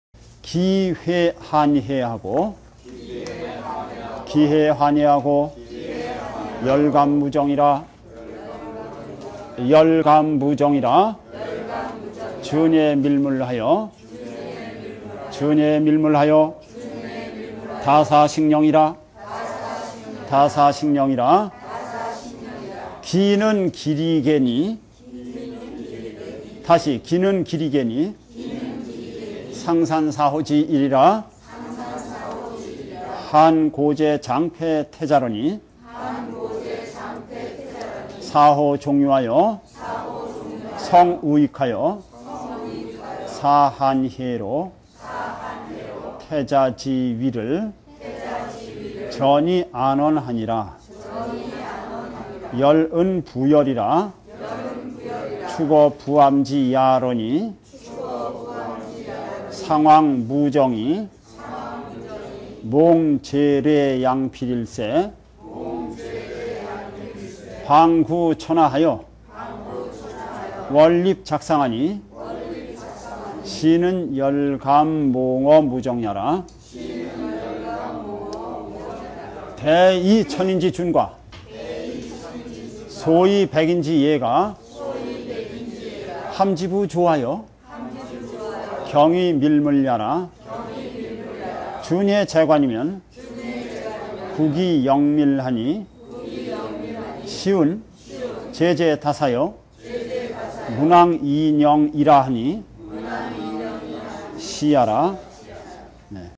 070 綺 回 漢 惠하고 說 感 武 丁하니라 -- 071 俊 乂 密 勿하여 多 士 寔 寧이라
綺는 綺里季니 商山四皓之一이라 漢高帝將廢太子러니 四皓從游하여 成羽翼하여 使漢惠로 太子之位轉而安焉하니라 說은 傅說이라 築於傅巖之野러니 商王武丁이 夢帝賚良弼일새 旁求天下하여爰立作相하니 是는 說感夢於丁也라

大而千人之俊과 小而百人之乂가 咸集于朝하여 經緯密勿也라 俊乂在官이면 國以寧謐하니 詩云 濟濟多士 文王以寧이 是也라